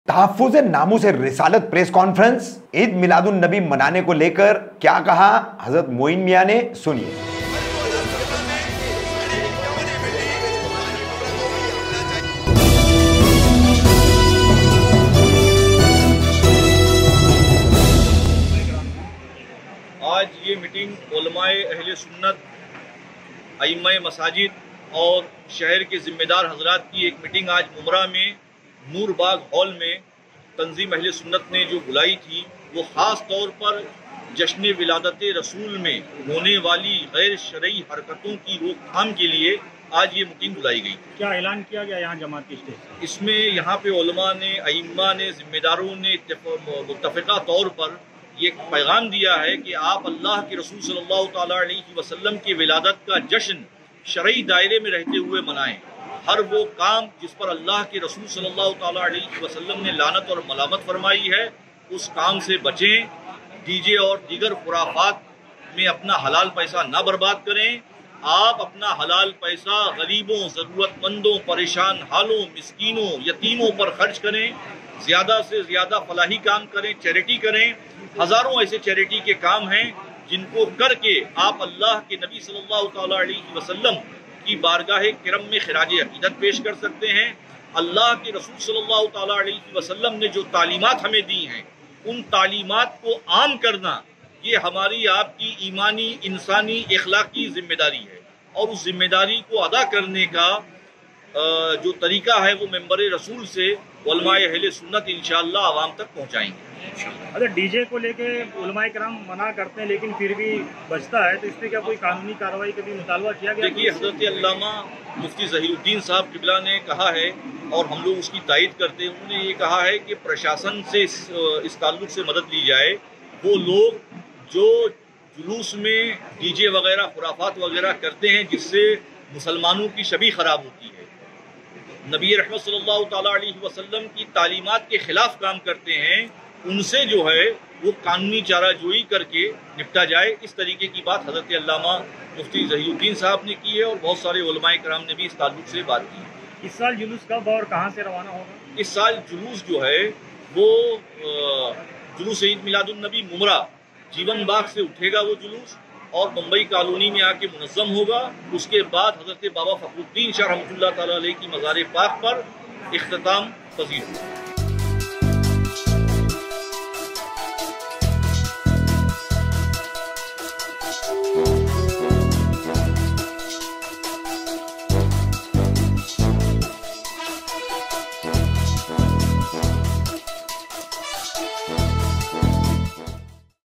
ईदमिलादुन्नबी मनाने को लेकर क्या कहा हजरत मोईनमियां ने सुनिये | Hindustani Reporter
ईदमिलादुन्नबी मनाने को लेकर क्या कहा हजरत मोईनमियां ने सुनिये

गैरशरई कामों से बचने की अपील